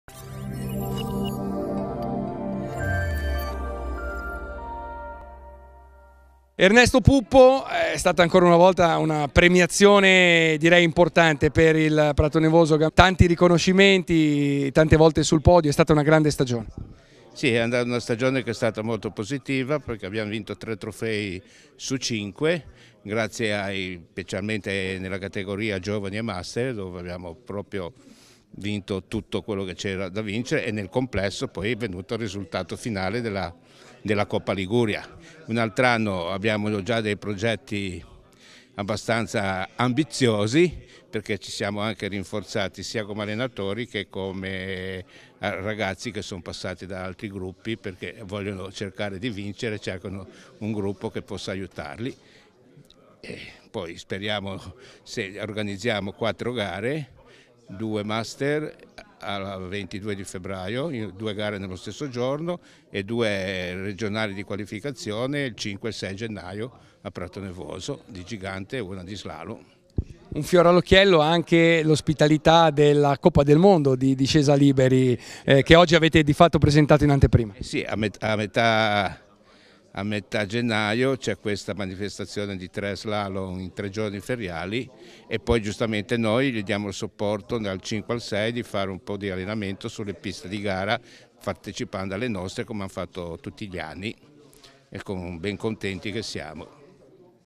Pratonevoso Gam Genova vince la Coppa Liguria 2018-2019
Ernesto Puppo, presidente Pratonevoso Gam Genova, esprime la sua soddisfazione per i risultati agonistici stagionali, determinanti per la conquista della Coppa Liguria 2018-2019 dedicata allo storico presidente Giancarlo Gaminara, e parla anche dell'impegno organizzativo dei prossimi appuntamenti della stagione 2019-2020.